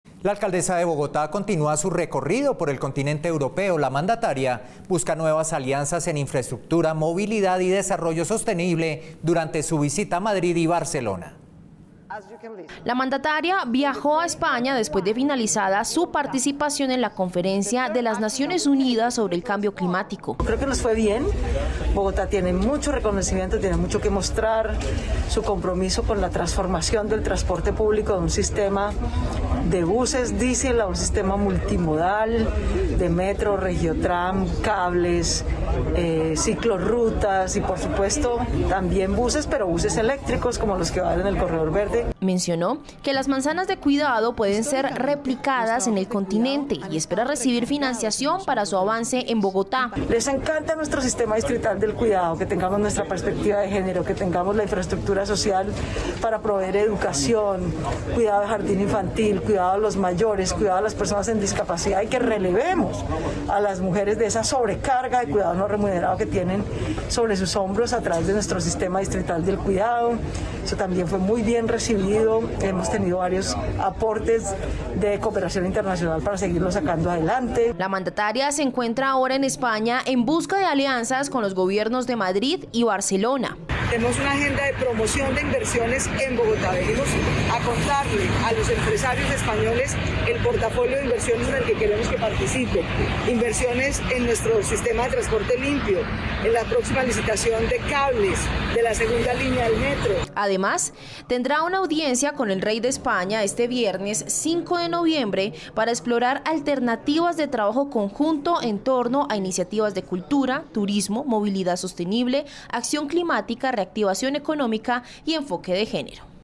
Claudia López visita España en busca de nuevas alianzas con gobiernos de Barcelona y Madrid
NoticiasCapital | La alcaldesa Claudia López continúa su recorrido por el continente europeo. La mandataria busca nuevas alianzas en infraestructura, movilidad y desarrollo sostenible durante su visita a Madrid y Barcelona.